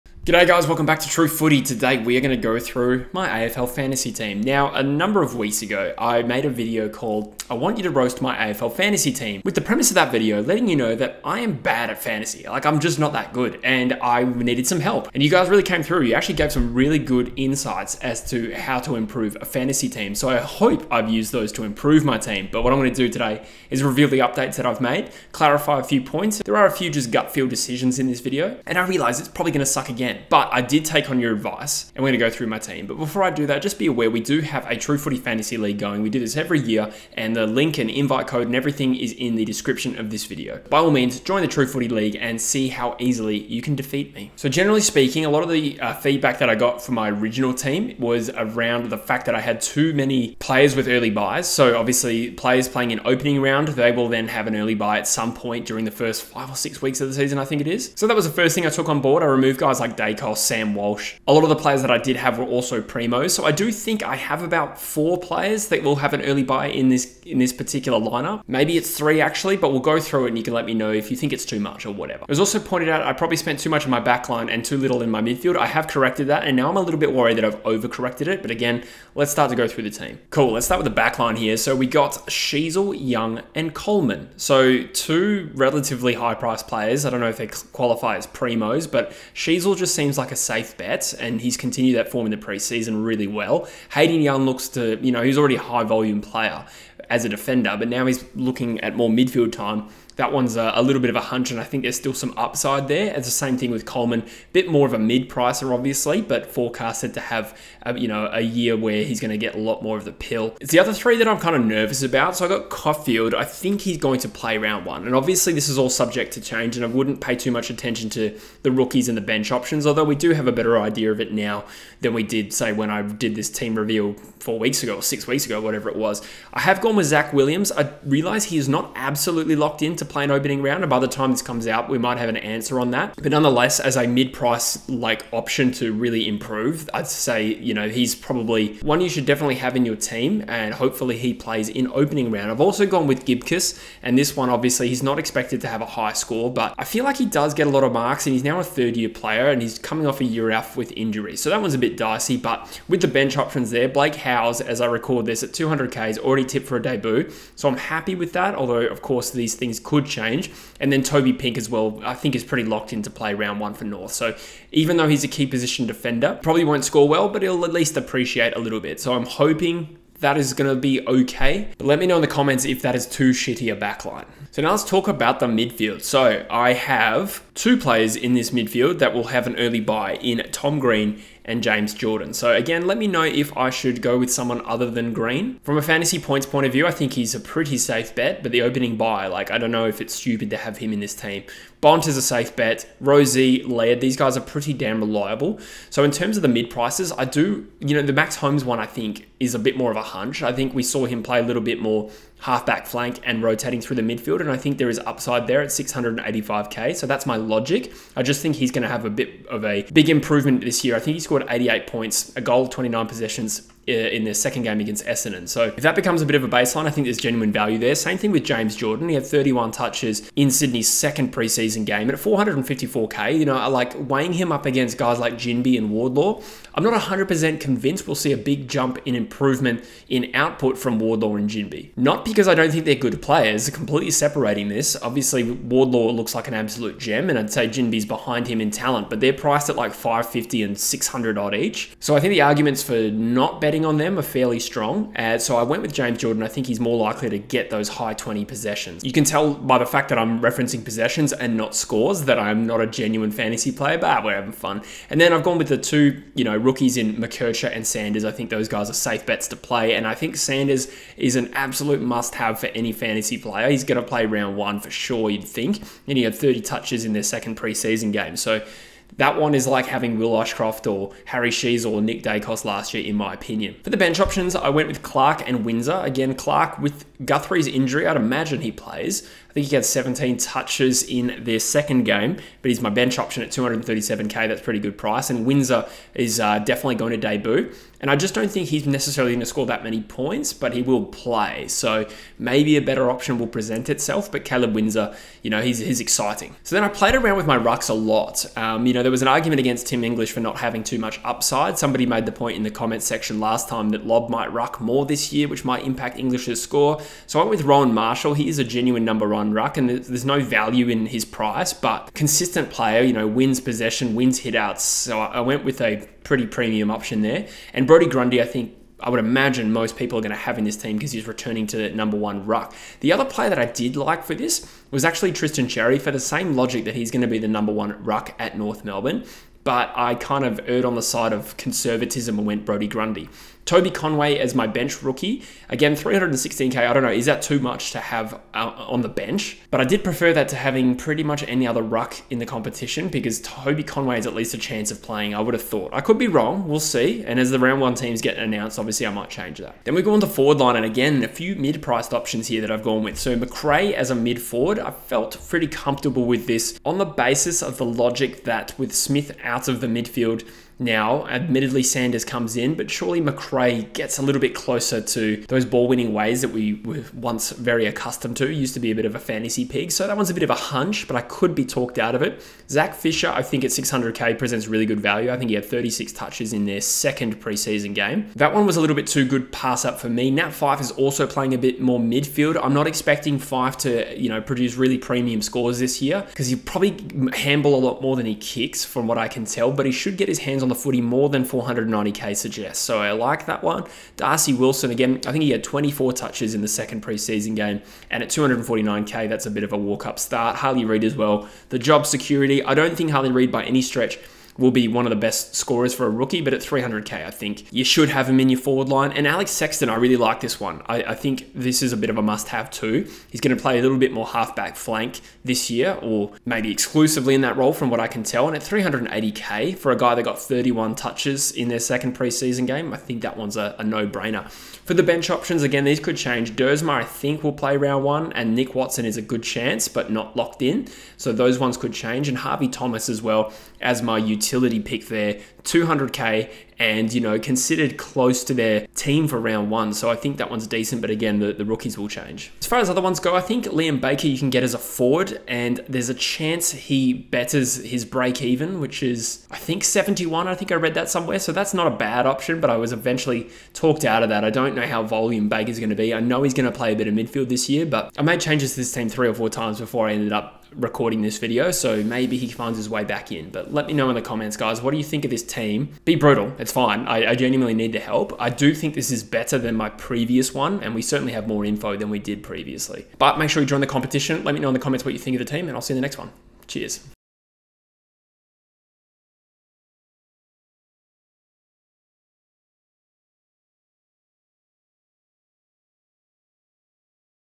Revealing my AFL Fantasy team for 2024!
Jesse reveals his AFL Fantasy team for 2024 - and he needs your help.